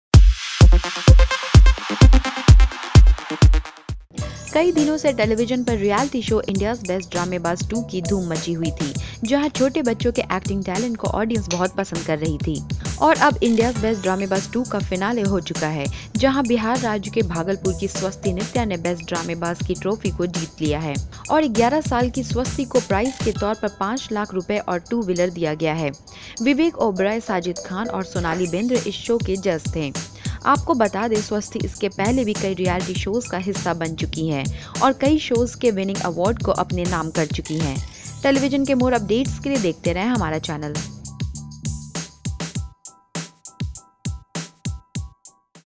Swasti Nitya Won The Title of India’s Best Dramebaaz 2 !
kai dino se television par reality show India’sBest Dramebaaz 2 ki dhum machi hui thi jahan chote bachon ke acting talent ko audience bht pasand kar rahi thi..aur ab India’s Best Dramebaaz 2 ka finale ho chukka hai jahan Bihar rajya ke Bhagalpur ki Swasti Nitya ne best dramebaaz ki trophy ko jeet liya hai..aur 11 saal ki Swasti ko prize ke taur par paanch lakh rupye aur two wheeler diya gaya hai.. Vivek oberoi, Saajid khan aur Sonali bender is show ke judge the.. aapko bata de Swasti  iske pehle bhi kai reality showz ka hissa ban chuki hai..aur kai showz ke khitab ko bhi jeet chuki hai..television ke more updates ke liye dekhte rahe hamara channel..